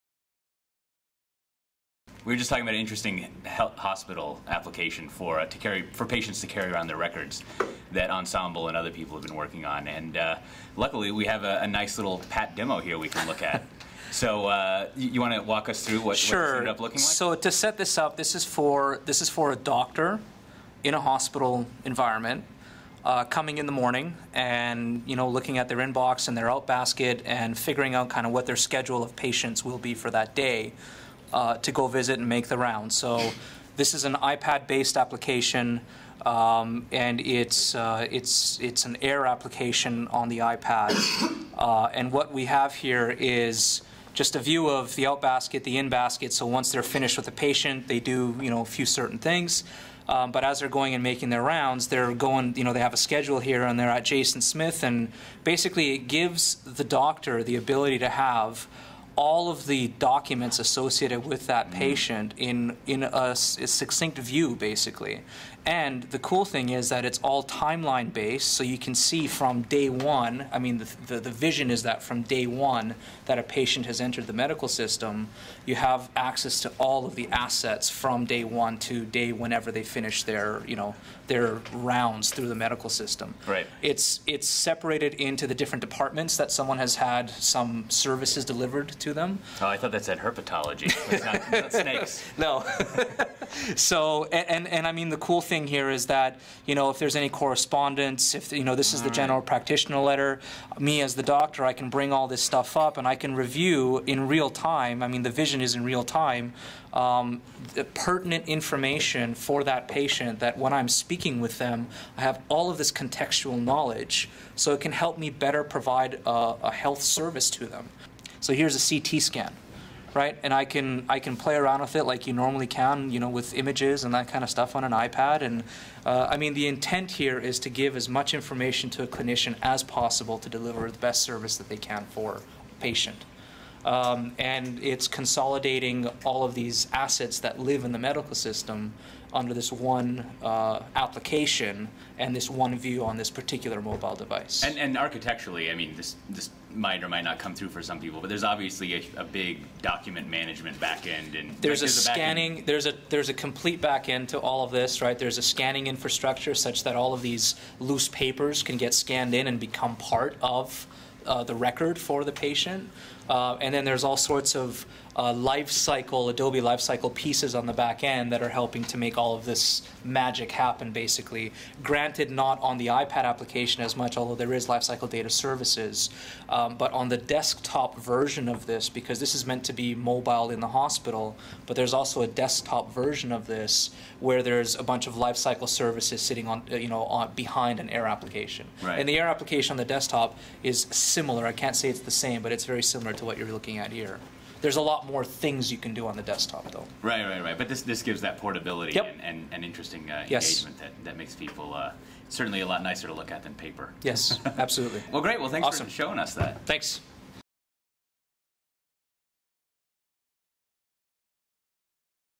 A medical Enterprise RIA demo - Vlad Ghelesel
There's few enterprise-y scenarios more fraught with pit-falls than converting the medical industry over to paperless, getting rid off all those paper and pen forms doctors, nurses, and hospital staff seem to have a tragic romance with. Thanks to the sheer beauty of new form factors, like the iPad, the folks at Ensemble have been finding success using enterprise RIA as the way to digitize medical records. Ensemble's Vlad Ghelesel shows us a demo of Ensembles approach to solving this problem.